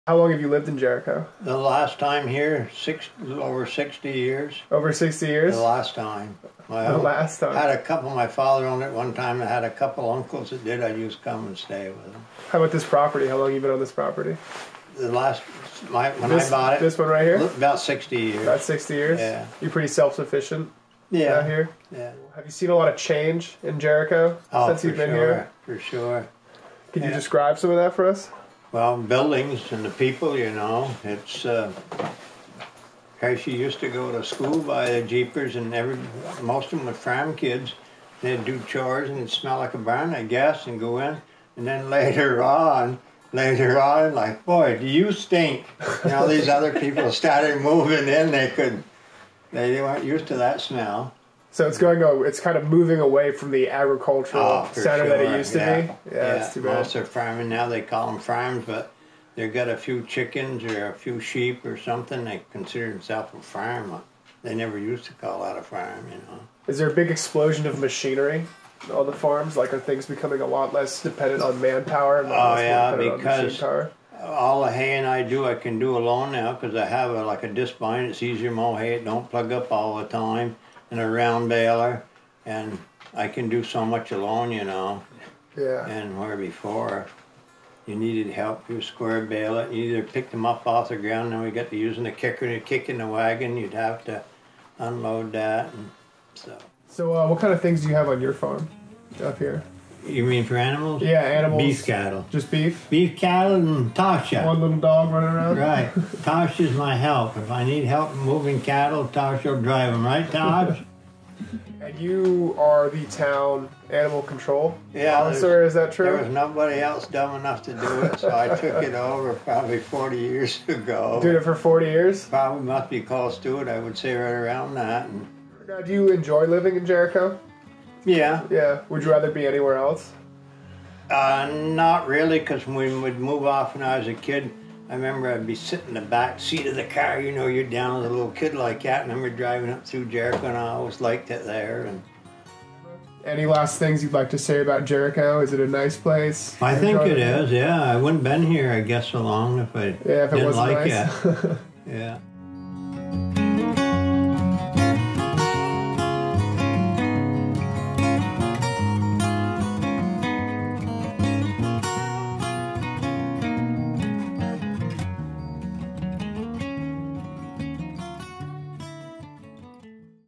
Jericho | Vermont Towns
Students at the University of Vermont conducted small case studies on rural communities in Vermont. This video features local resident Ed Stygels of Jericho, Vermont, who has lived in the town for 60 years.